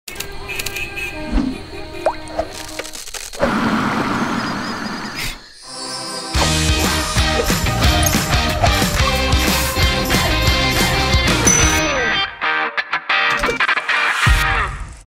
Наступил сезон отключения горячей воды? Сухой шампунь Batiste идет на помощь!
Отключение горячей воды уже вот-вот настанет? Ты устала греть воду в кастрюльке, чтобы помыть голову? Выход есть! Сухой шампунь Batiste уберёт жир у корней волос и освежит их всего за пару мгновений. Волосы снова выглядят чистыми и объемными ✨💁‍♀️ И не нужно никакой воды!